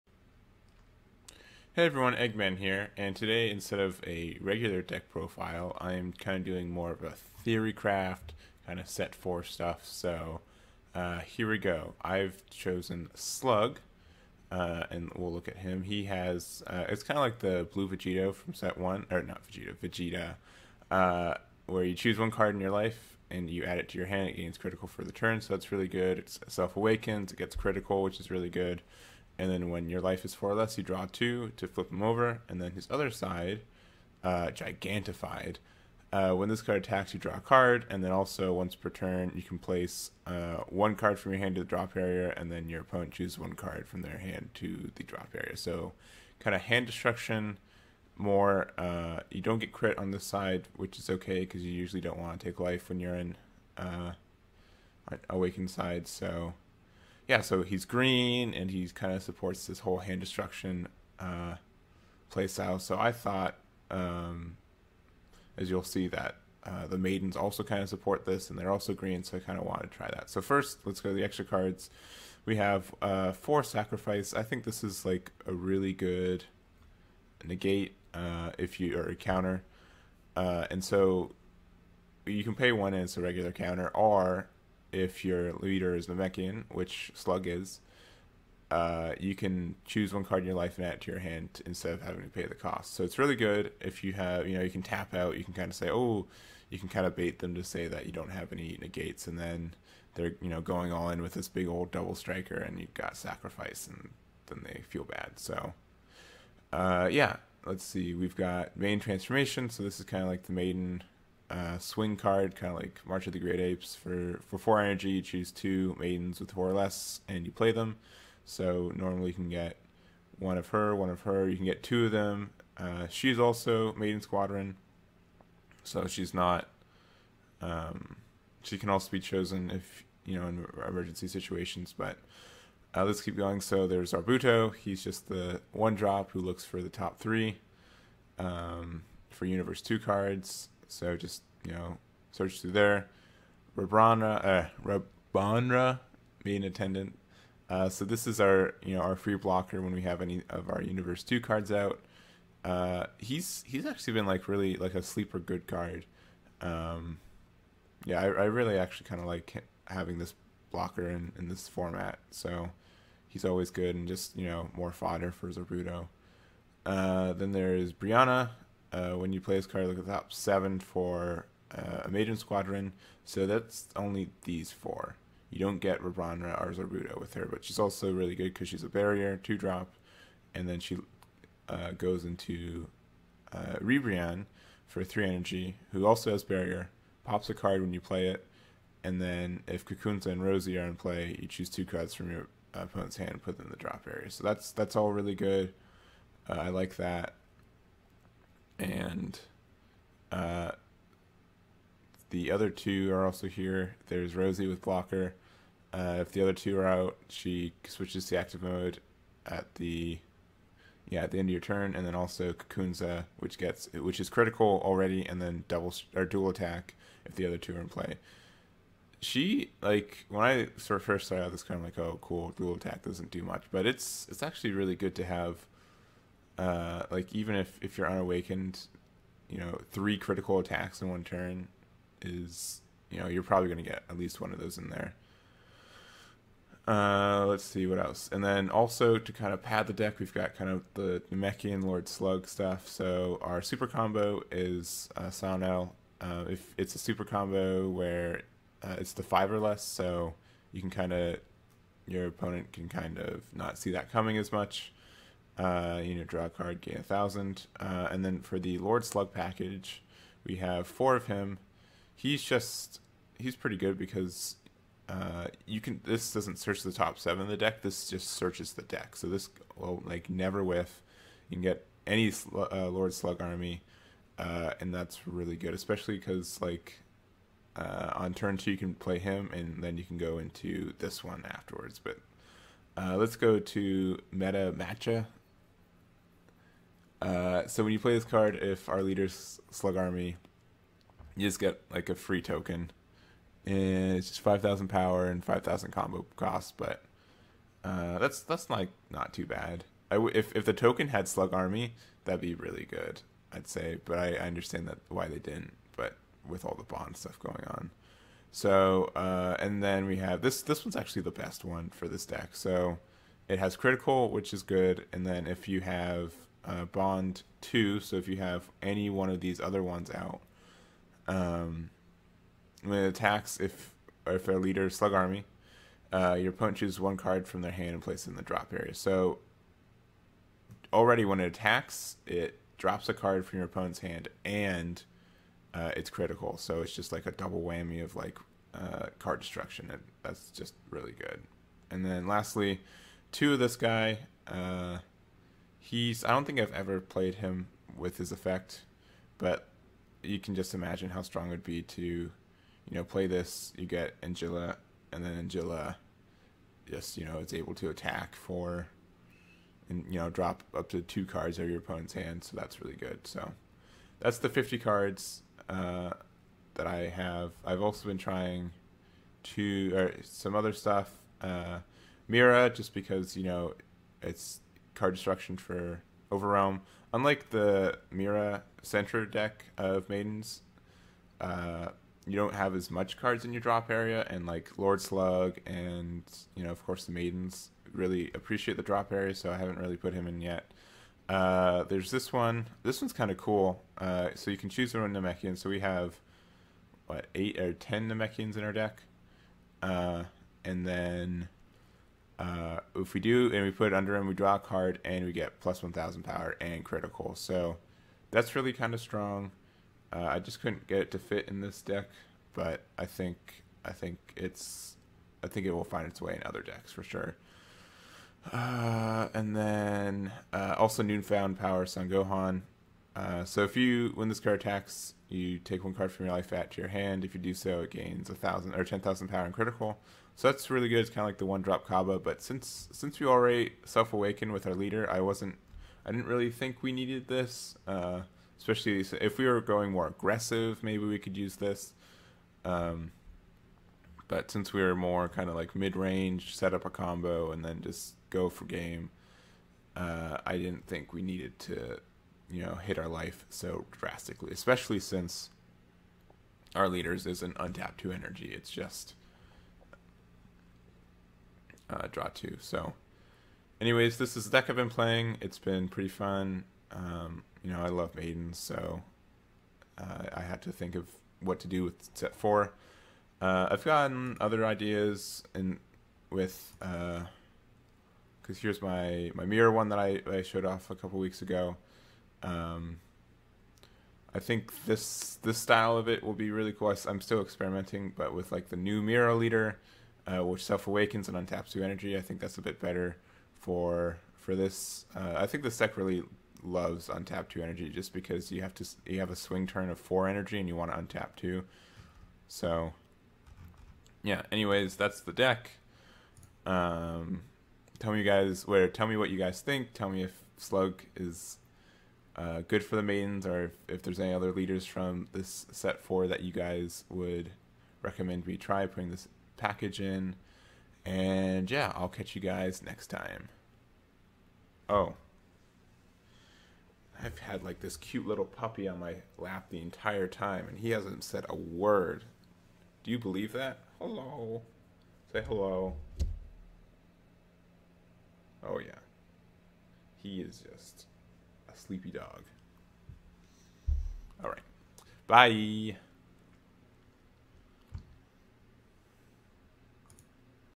DBS Card Game Deck Theorycraft: Slug Maidens
My first deck profile/theorycraft for Set 4. It takes my currently favorite package, Maidens, and tries to use it around one of the newest leaders, Slug! Can you imagine him in a maiden outfit?

I've started a Let's Play Channel, go check it out!